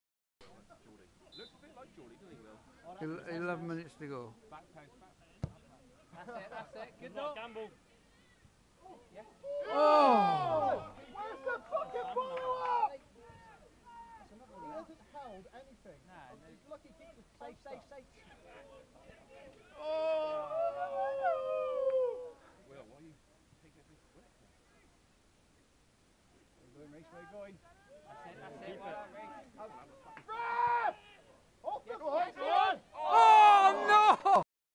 C.C,F.A Bond Timber Junior Cup Semi Final , St Stephen 1st 3 v 1 Wendron FC after xtra time.
match highlights A Luke Damerell free-kick is headed goalbound by Jordie Willmott the keeper saves, the ball is cleared then Courtney Rowe is put through but puts his shot wide.